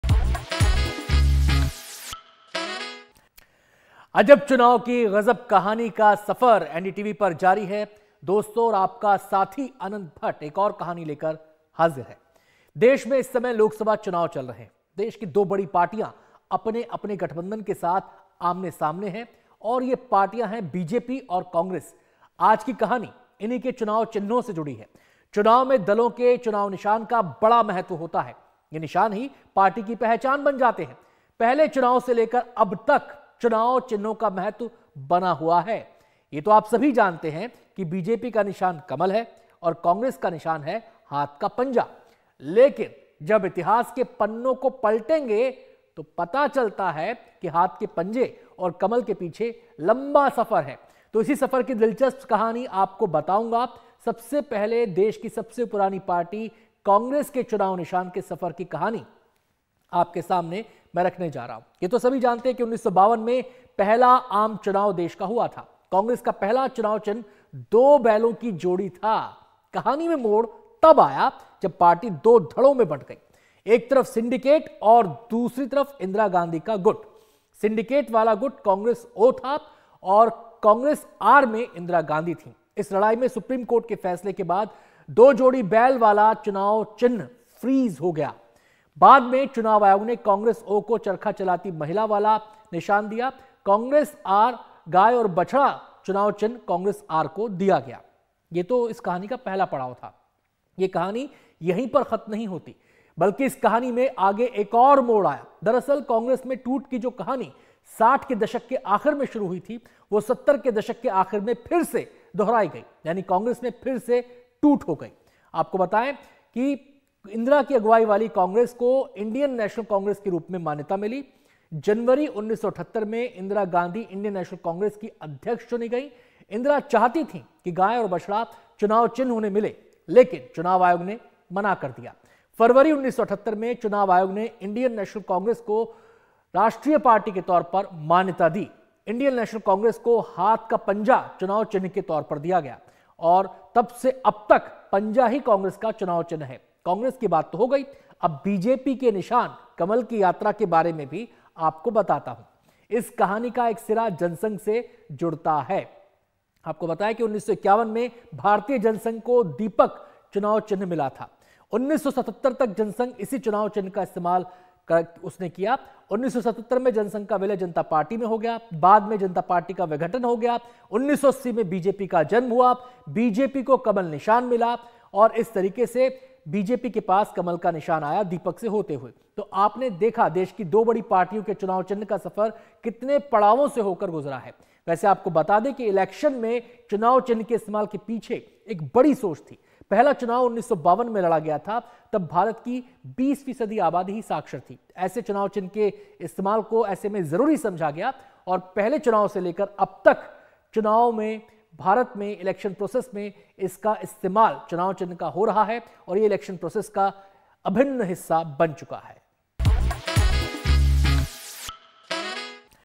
Congress और BJP की पहचान कैसे बने हाथ और कमल के निशान? चुनाव चिन्ह की दिलचस्प कहानी | Elections 2024
Ajab Chunaav Ki Gajab Kahaani: पहले चुनाव से लेकर अब तक भारत में इलेक्शन के लिए चुनाव चिन्हों का इस्तेमाल हो रहा है, अलग-अलग पार्टियों को अलग-अलग चुनाव चिन्ह जारी किए जाते हैं. समय के साथ ये चुनाव चिन्ह ही किसी पार्टी की पहचान बन जाते हैं. आज हाथ का पंजा और कमल का निशान कांग्रेस (Congress) और बीजेपी (BJP) की पहचान बन चुके हैं, लेकिन दोनों पार्टियों के निशान अलग-अलग पड़ावों के बाद दोनों पार्टियों को मिले हैं.